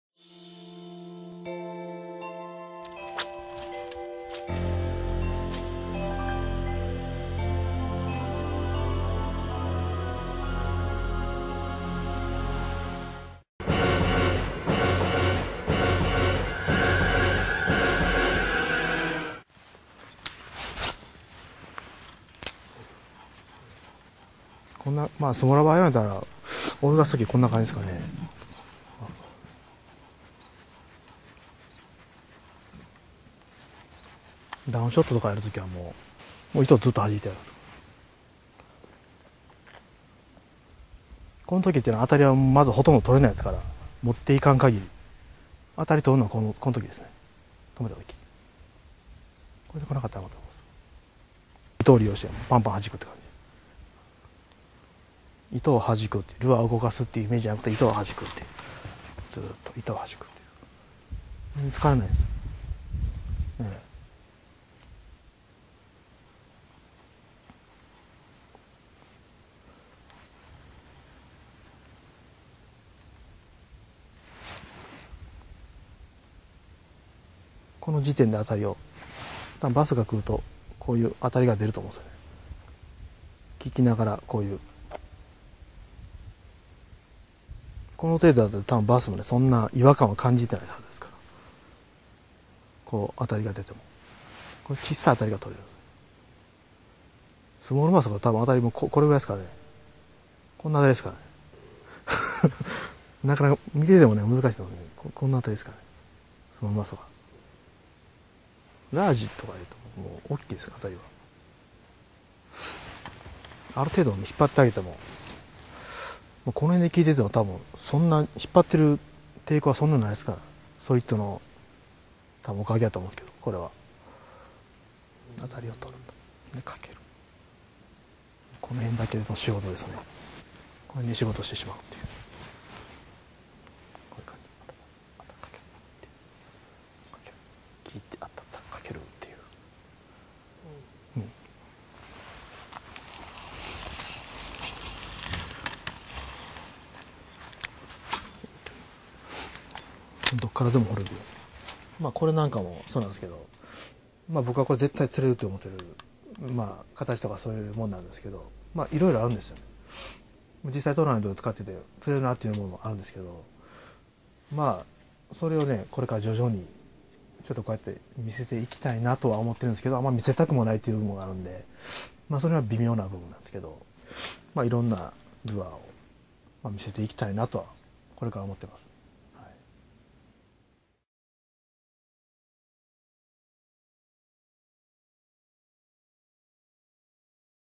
福島健と秘密の部屋 超人的喰わせ福島的ロッドワークの秘密
2011年JBワールドチャンピオン福島健の超人的喰わせ
福島的ロッドワークの秘密